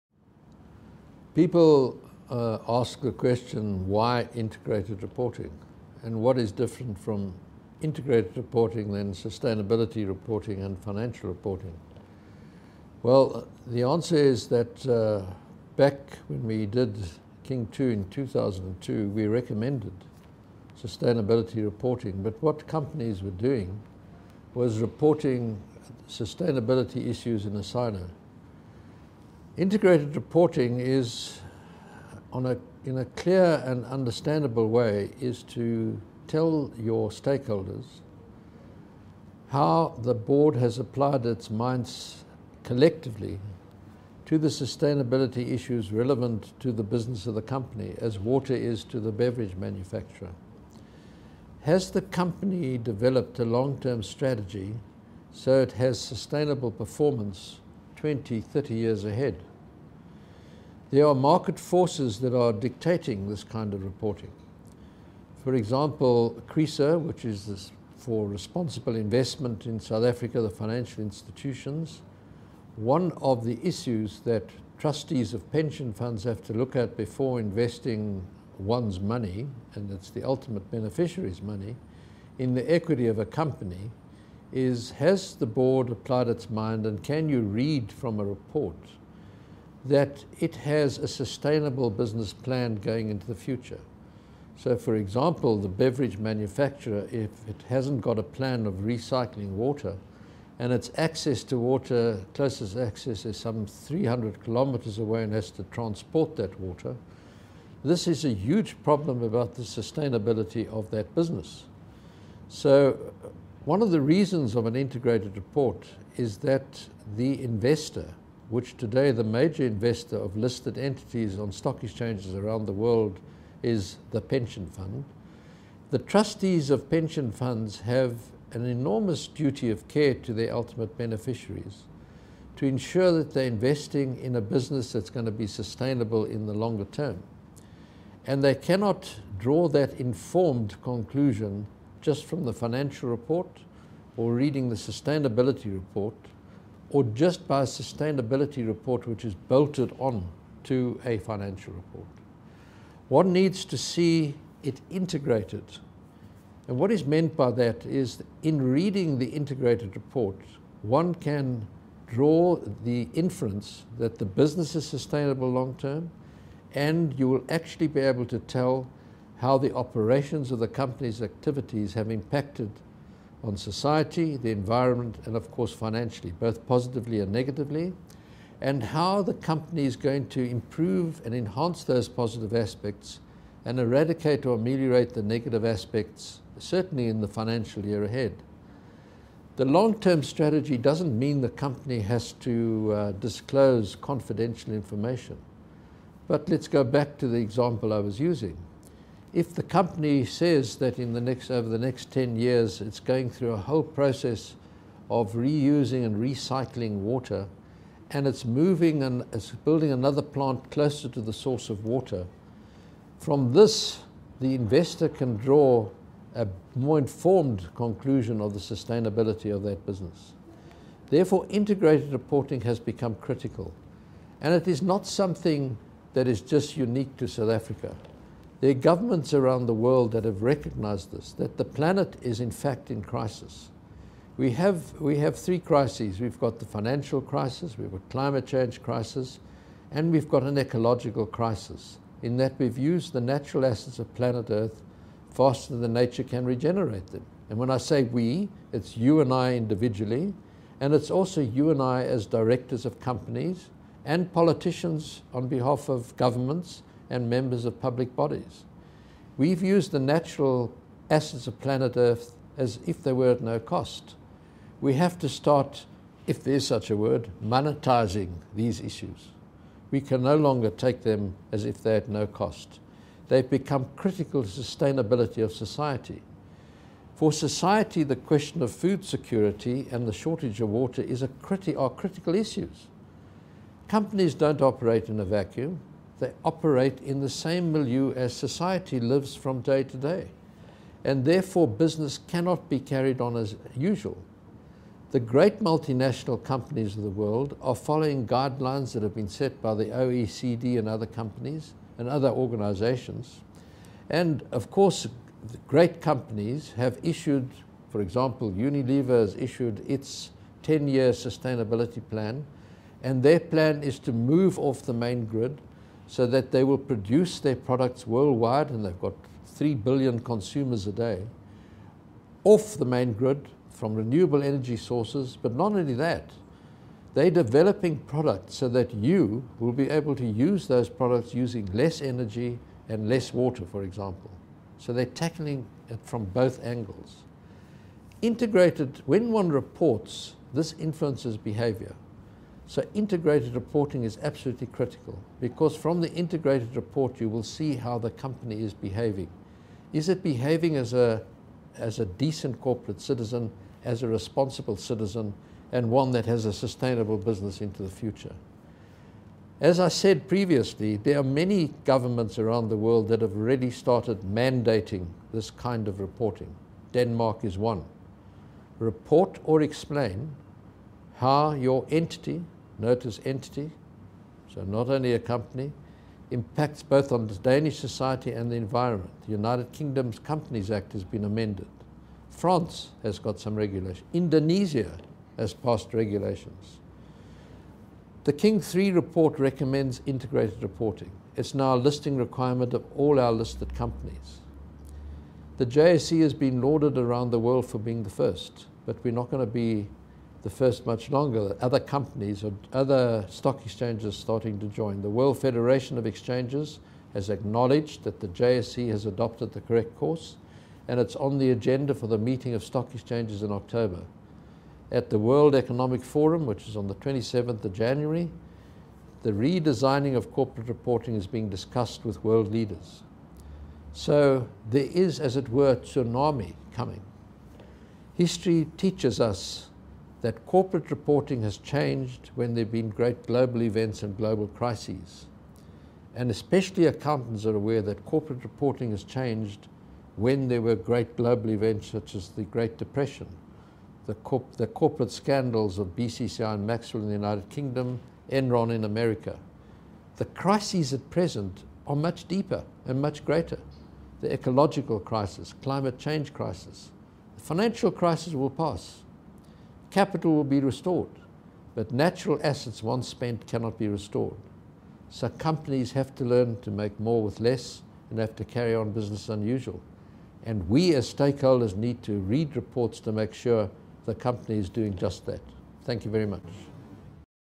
Prof. Mervyn King explains Integrated Reporting for South Africa - Part 1
Professor Mervyn King explains Integrated Reporting, at the launch of South Africa's new Framework for Integrated Reporting and the Integrated Report Discussion Paper on 25 January 2011.

South Africa is the first country to issue specific guidelines defining the content of an integrated report for listed companies. Integrated Reporting is now a stock exchange listing requirement for South African companies and will replace the old form of annual report which focused primarily on financial information and the short-term horizon.

An integrated report does not simply bolt the sustainability report to the financial report. It incorporates, in clear language, material information from these and other sources to enable stakeholders to evaluate an organisation's performance and to make an informed assessment about its ability to create and sustain value.